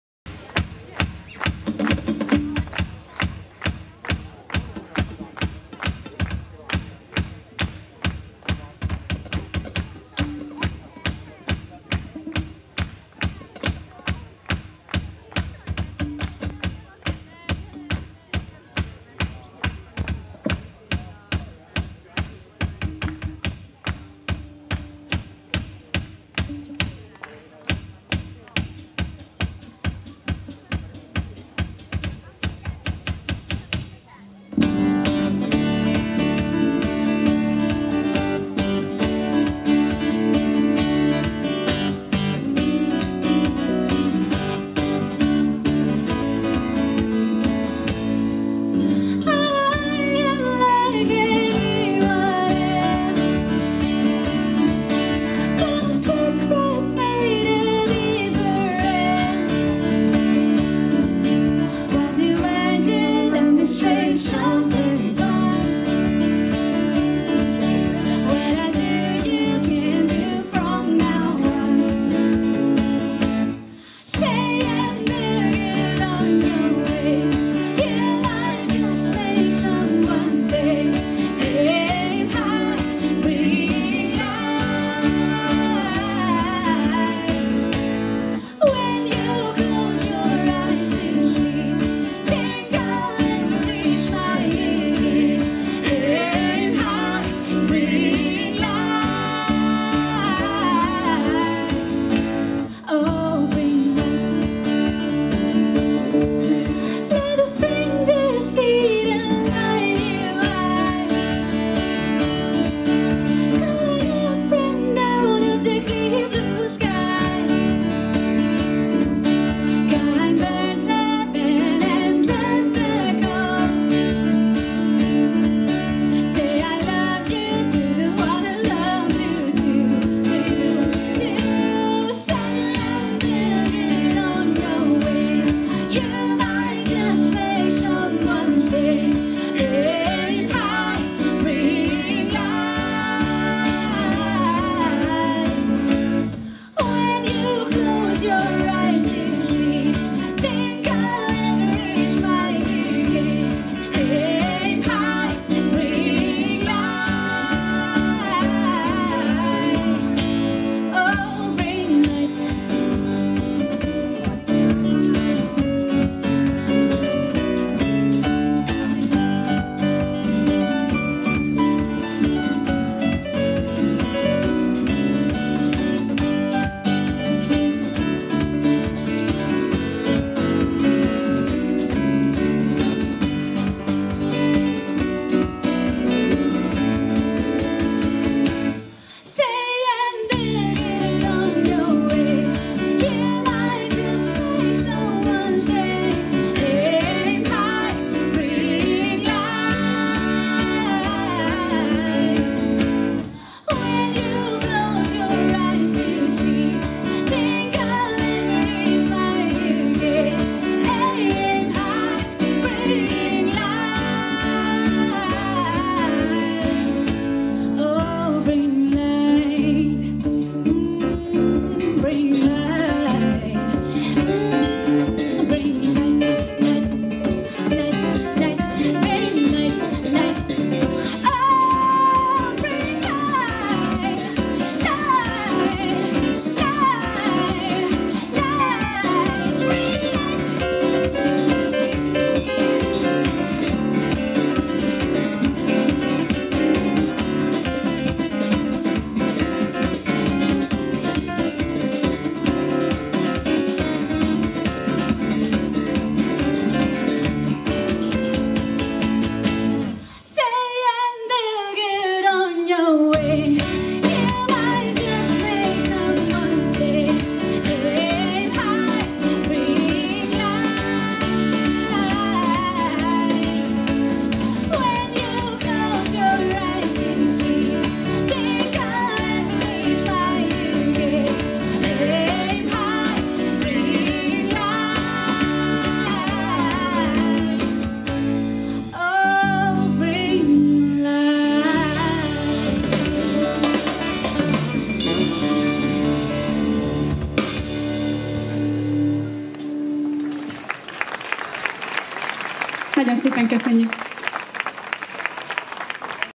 Boggie (Csemer Boglárka) koncert 3 -2017 Siófok Bor és Kenyér Ünnepe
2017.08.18.-án Siófokon a Bor és Kenyér Ünnepe Fesztiválon Boggie (Csemer Boglárka) koncetjéből részlet.
(A felvétel Panasonic Lumix G80H (14-140mm objektív) MILC fényképezőgéppel készült.)